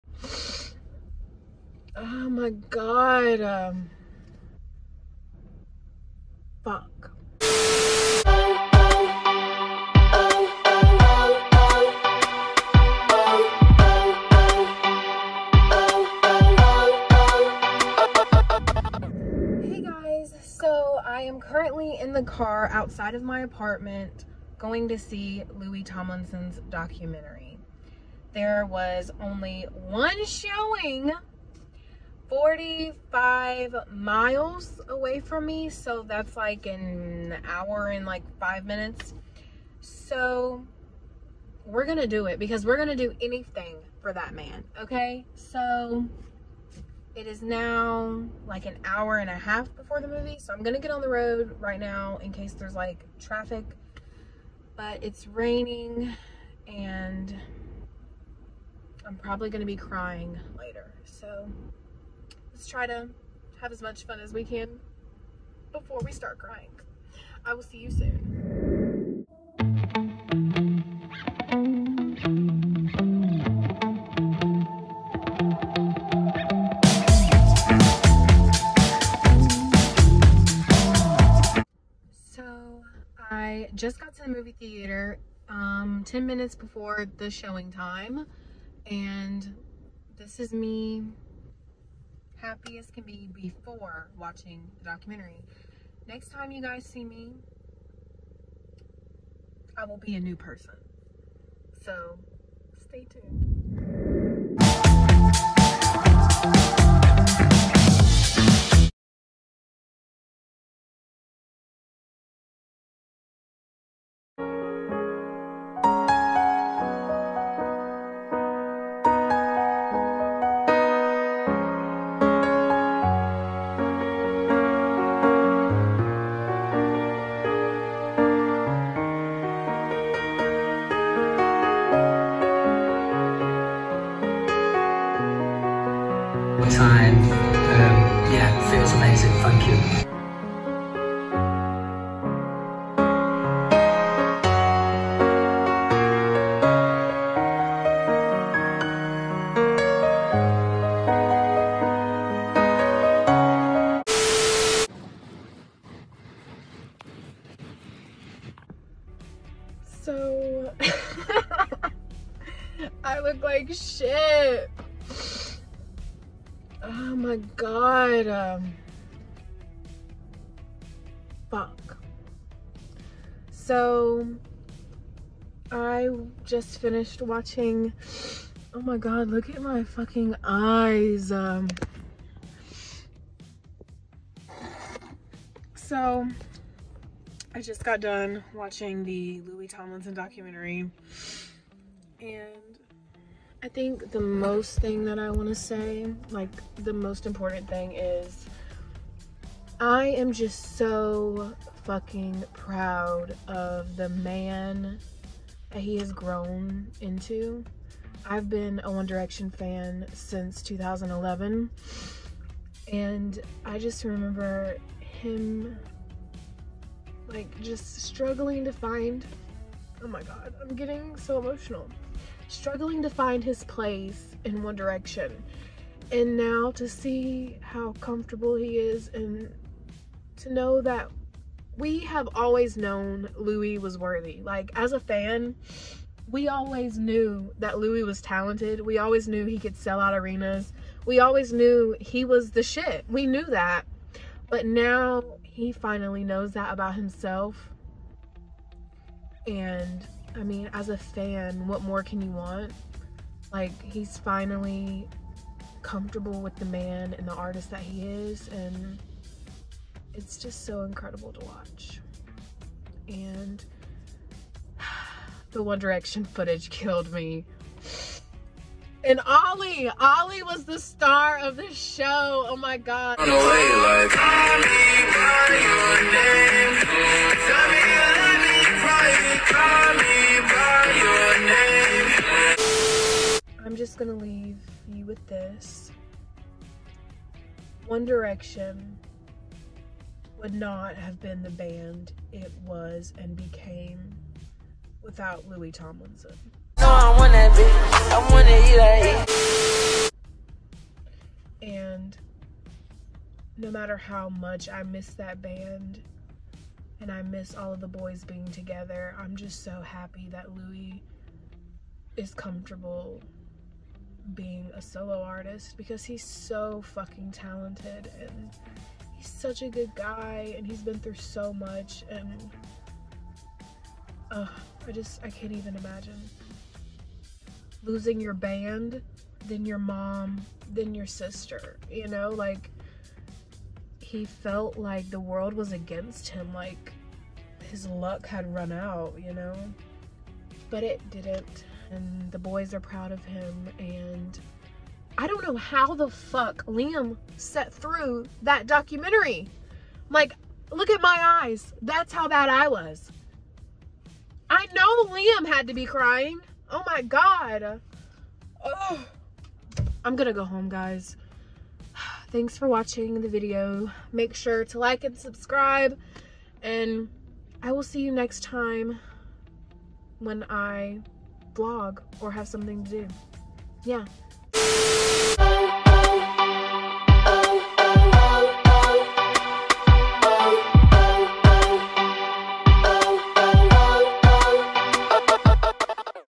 VLOG #1: All of those voices - Louis Tomlinson
Come with me to see Louis Tomlinson’s new documentary, All of those voices!

All of those voices: An intimate and unvarnished look at the life and career of British pop singer Louis Tomlinson, with never-before-seen footage and behind-the-scenes access to his sold-out 2022 world tour.

Director: Charlie Lightening